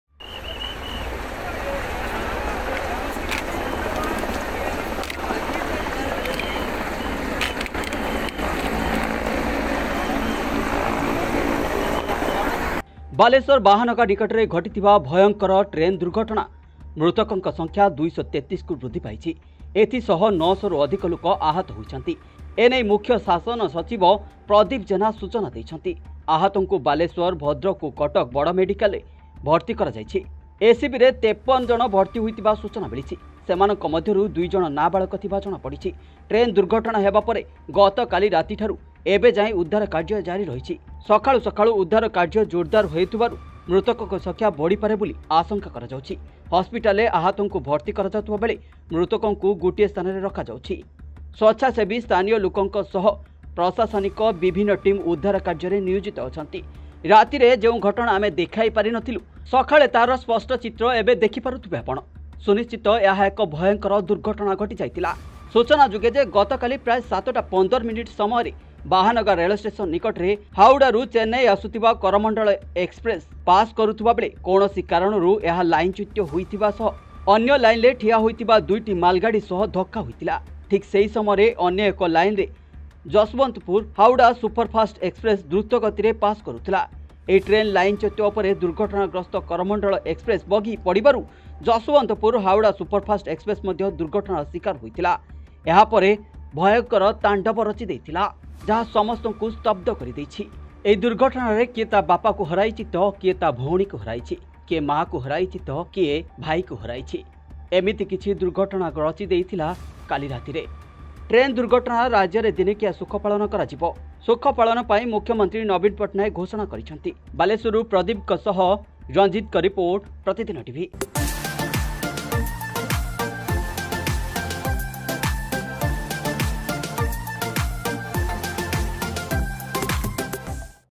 ଟ୍ରେନ୍‌ ଦୁର୍ଘଟଣାରେ ମୃତ୍ୟୁ ସଂଖ୍ୟା ୨୩୩କୁ ବୃଦ୍ଧି ୯୦୦ରୁ ଅଧିକ ଆହତ ICoromandel train accident update.
ବାଲେଶ୍ୱର ବାହାନଗା ଭୟଙ୍କର ଟ୍ରେନ୍‌ ଦୁର୍ଘଟଣାରେ ମୃତ୍ୟୁ ସଂଖ୍ୟା ୨୩୩କୁ ବୃଦ୍ଧି ୯୦୦ରୁ ଅଧିକ ଆହତ II#PratidinTv